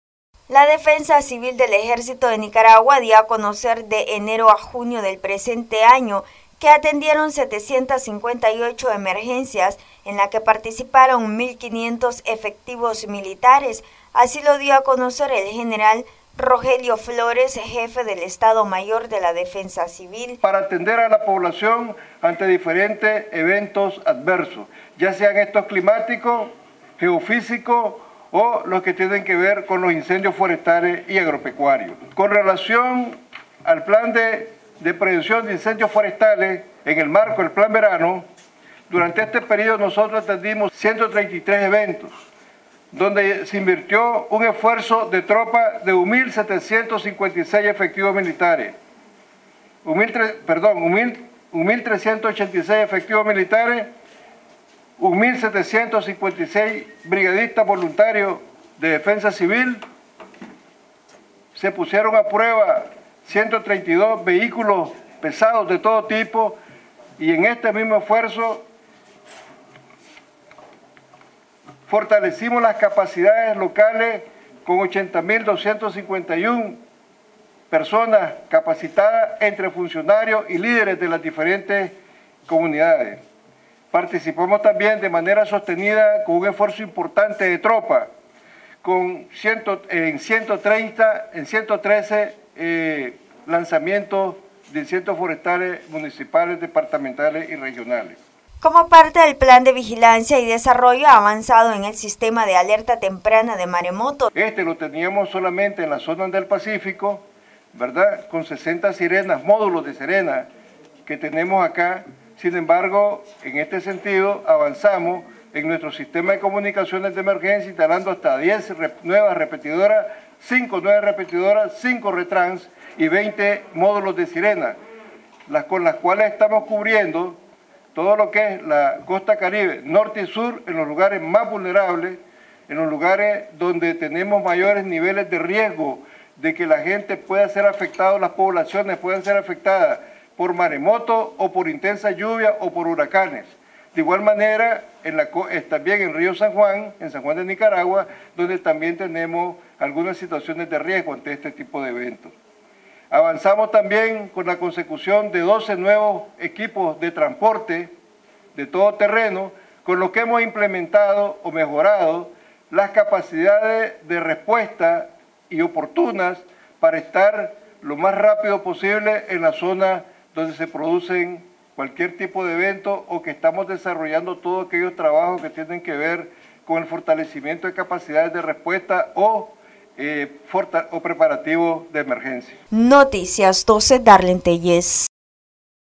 Defensa Civil de enero a junio del presente año atendió 758 emergencias
Noticias12Nicaragua En el marco del 43 aniversario de fundación del ejército de Nicaragua, Defensa Civil dio a conocer que de enero a junio del presente año atendieron 758 emergencias.